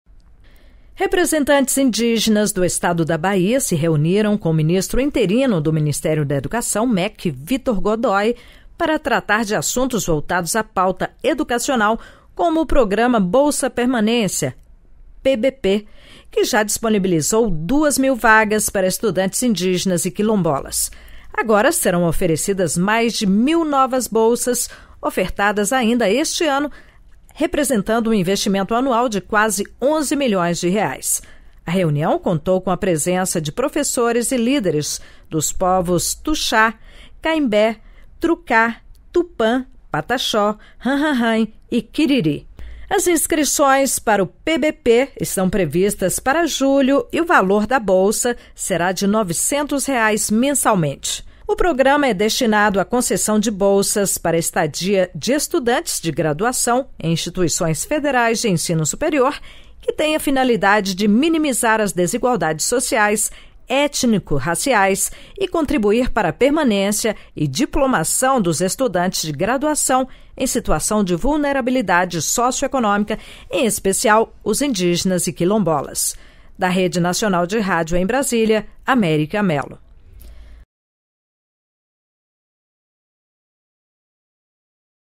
08/04/2022 - INDÍGENAS DA BAHIA SE REÚNEM PARA TRATAR DO BOLSA PERMANÊNCIA
Serão oferecidas mais mil novas bolsas, ofertadas ainda este ano, representando um investimento anual de quase R$ 11 milhões.

Representantes indígenas do estado da Bahia, se reuniram com o ministro interino do Ministério da Educação (MEC), Victor Godoy, para tratar de assuntos voltados à pauta educacional, como o Programa Bolsa Permanência (PBP), que já disponibilizou 2 mil vagas para estudantes indígenas e quilombolas.

Agora, serão oferecidas mais mil novas bolsas, ofertadas ainda este ano, representando um investimento anual de quase R$ 11 milhões.

A reunião contou com a presença de professores e líderes dos povos Tuxá, Kaimbé, Truká Tupã, Pataxó Hã-Hã-Hãe e Kiriri.

As inscrições para o PBP estão previstas para julho e o valor da bolsa será de R$ 900 mensalmente.

O Programa é destinado à concessão de bolsas para estadia de estudantes de graduação em instituições federais de ensino superior que têm a finalidade de minimizar as desigualdades sociais, étnico-raciais e contribuir para permanência e diplomação dos estudantes de graduação em situação de vulnerabilidade socioeconômica, em especial os indígenas e quilombolas.

xxxxxxxxxxx

registrado em